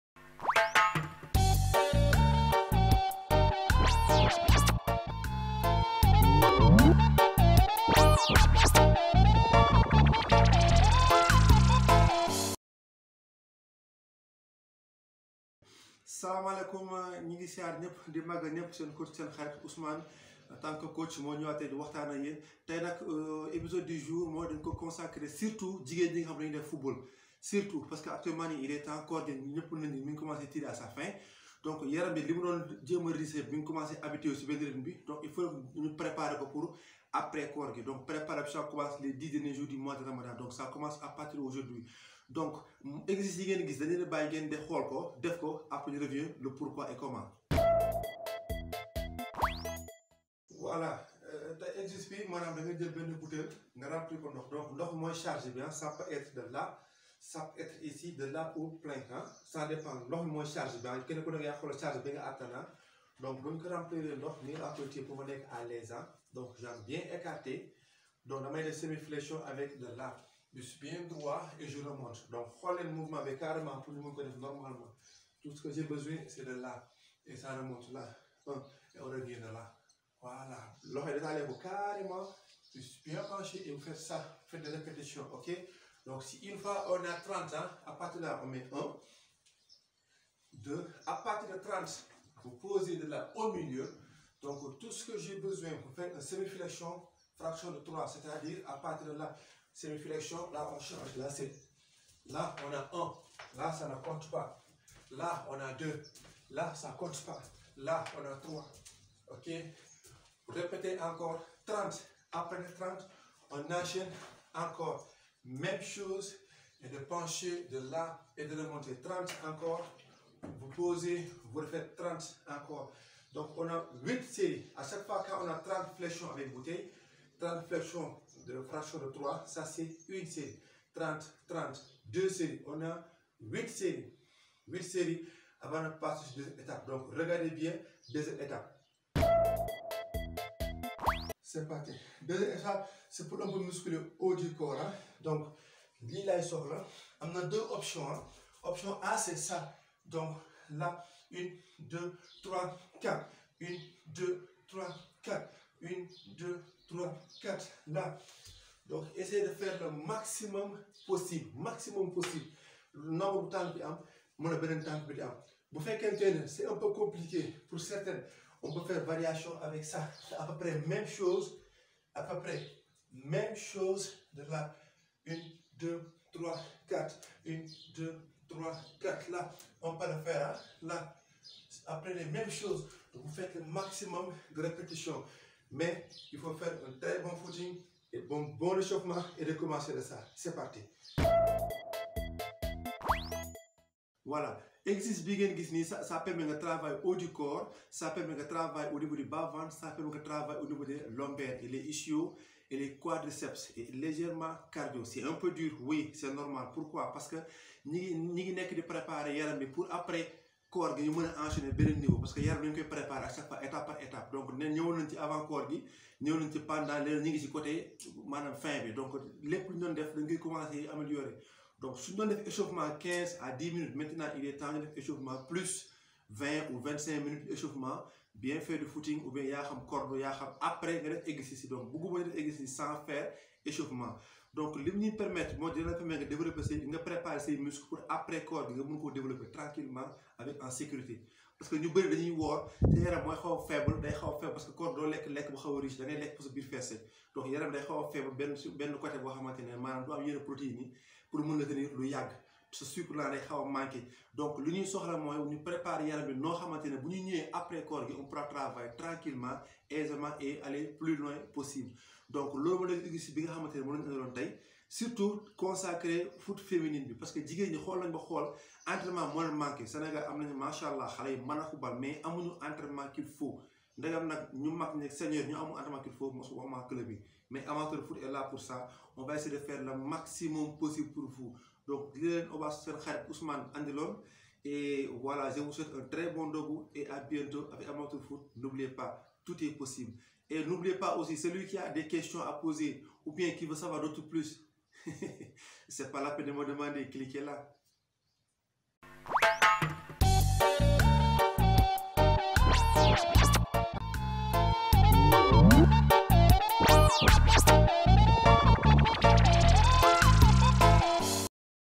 Confinement, Sport & Ramadan épisode 7
Suivez l'épisode 7 proposé par Coach Ousmane Sow, une nouvelle séance d'entrainement pendant cette période confinement, coronavirus, ramadan.